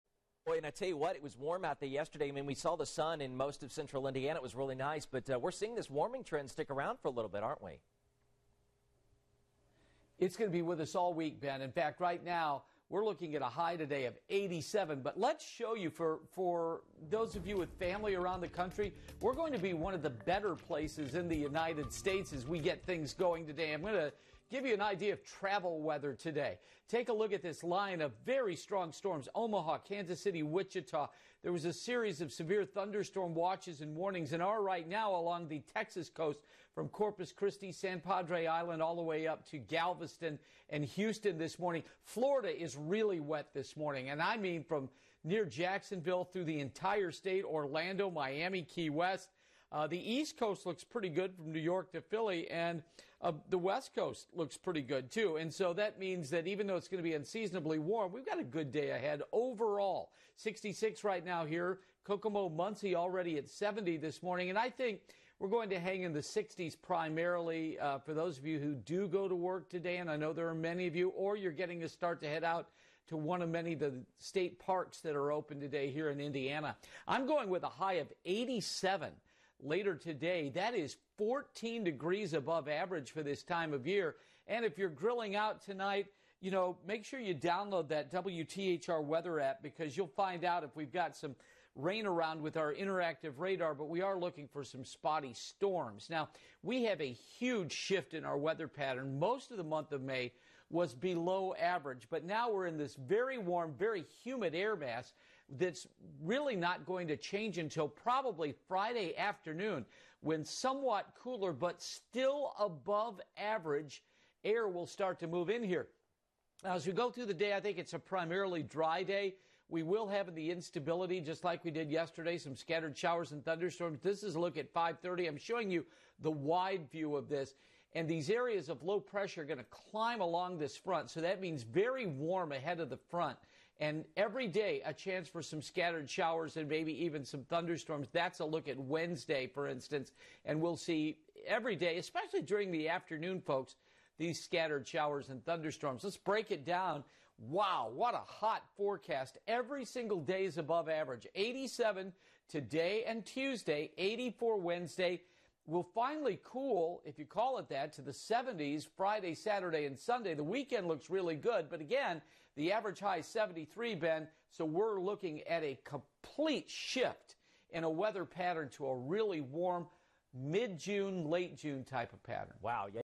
5-25-20 Sunrise Forecast
Memorial Day is finally here, and the heat that we tapped into Sunday will be even more pronounced today. Look for heat, more humidity and a chance for spotty thunderstorms this afternoon. Today, our high jumps to 87 degrees — a full 14 degrees warmer than average — and the heat will stay with us the rest of the week, making this the warmest period of 2020 yet.